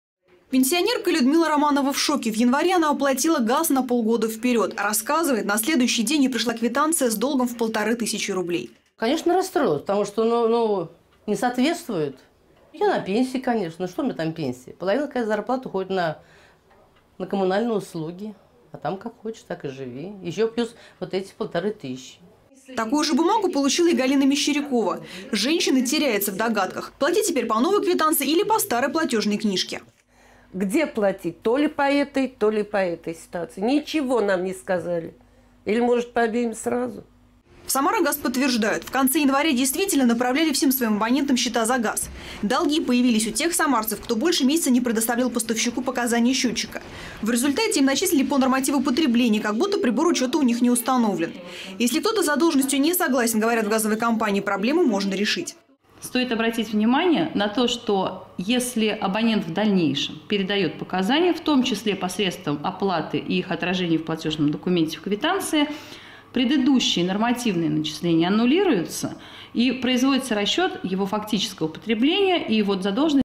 Ложная тревога.
Ошибка поставщика или сами виноваты? Жители Самары стали получать квитанции на оплату газа с неожиданными для них долгами - по несколько тысяч рублей. Причем чек многим выставили исходя из нормативного потребления, несмотря на установленные приборы учёта. В ситуации разбиралась наша съемочная группа.